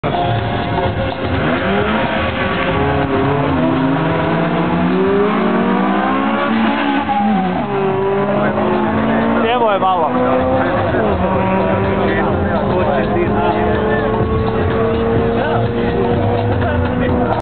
SR Civic vti vs. Opel Kadett
Practice run in Rijeka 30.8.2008, Honda Civic VTi vs. Opel Kadett